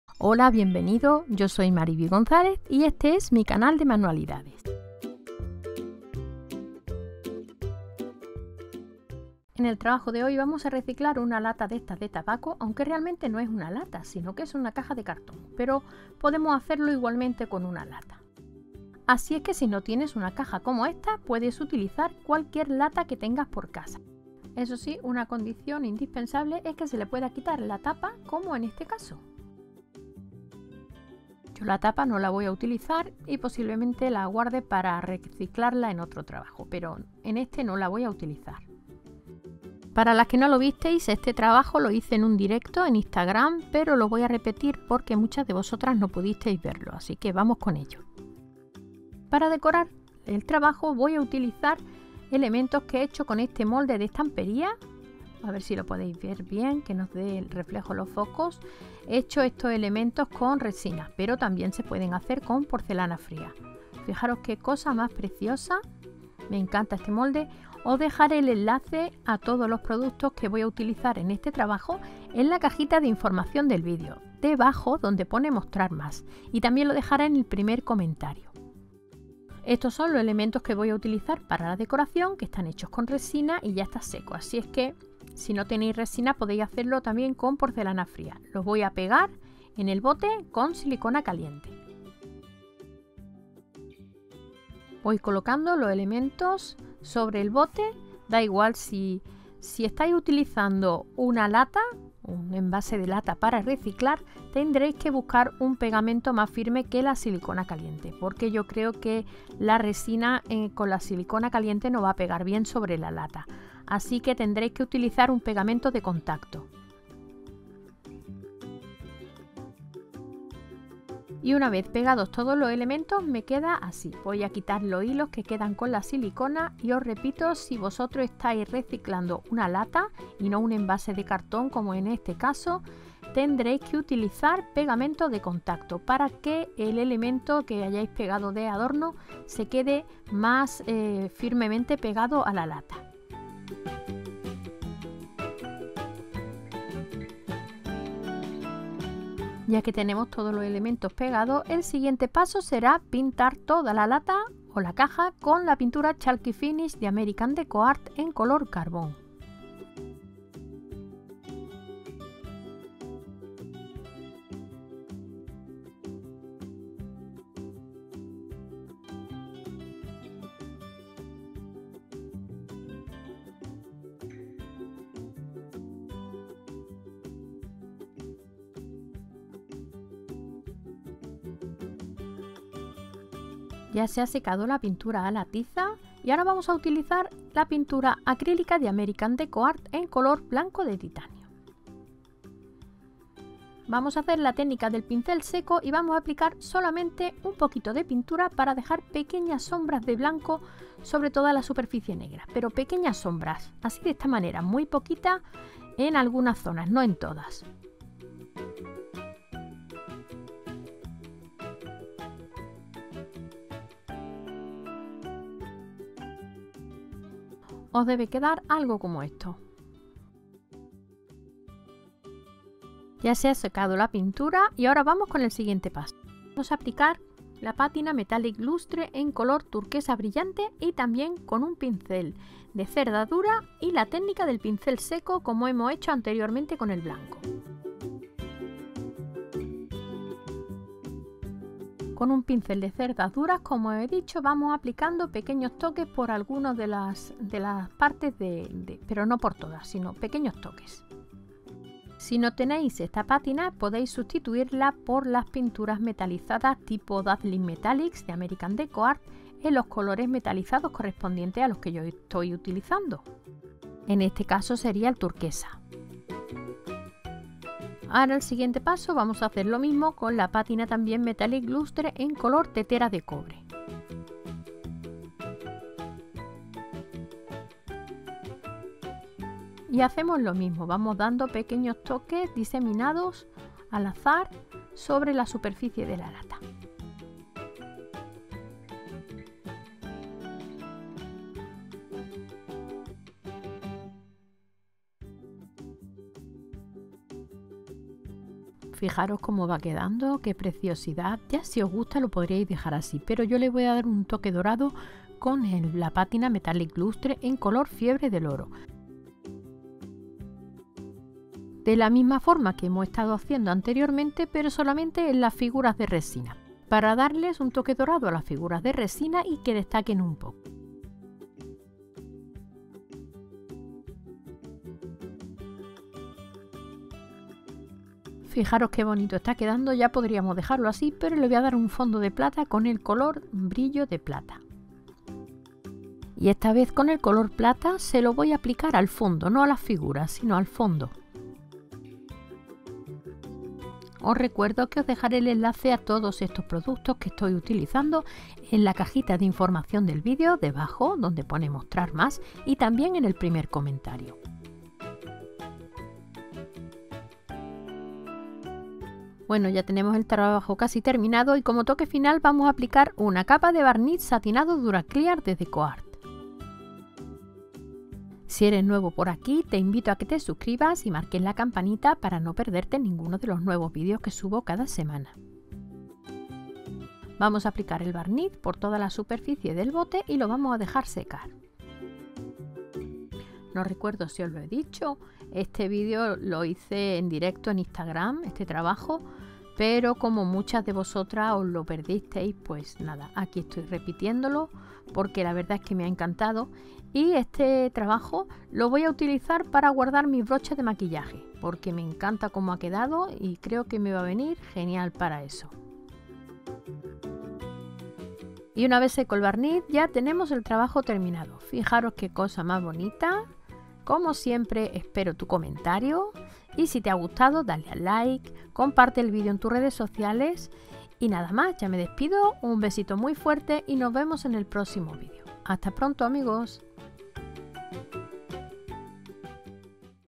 Caja decorada con efecto metal y DECOUPAGE muy facil Diy
* Metallic lustre Decoart colores turquesa brillante, tetera de cobre, fiebre del oro, y brillo de plata

C/Leon,3(POLIGONO IND.
Cabo Calleja
Fuenlabrada (MADRID), ES 28947
→MIS TIENDAS